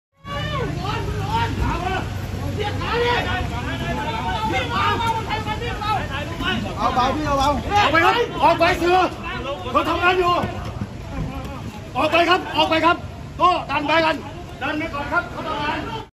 เจ้าหน้าที่จับผู้ชุมนุมที่ดินแดง 19 มิ ย 65
อัพเดทสถานการณ์ล่าสุด เมื่อเวลา 20.40 น. เจ้าหน้าที่คฝ. ได้จับชายไม่ทราบนามที่